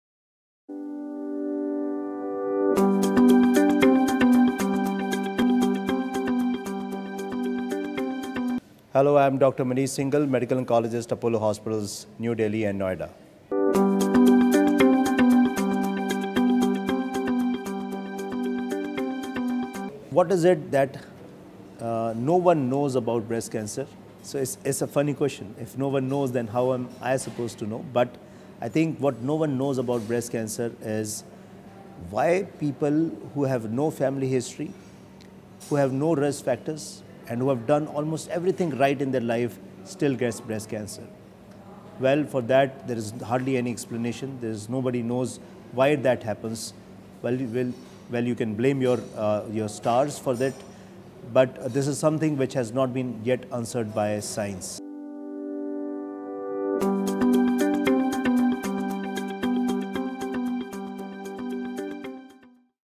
Things No One Knows About Breast Cancer | Unknown Breast Cancer Facts | Dr. Manish Singhal
There are few questions that people have about breast cancer but the doubts still remain the same. The most important one is, why do people with no family history still get affected by breast cancer? Or even with the right habits and healthy diet get affected by breast cancer? And some get cured but others don't.

The majority of women diagnosed with breast cancer have no close relatives with this disease. Regardless of family history, discuss mammography guidelines and schedule regular screenings with your health provider. The biggest risk factors for breast cancer are being a woman and growing older.

Breast cancer occurs mainly in women, but men can get it, too. Many people do not realize that men have breast tissue and that they can develop breast cancer. Cells in nearly any part of the body can become cancer and can spread to other areas.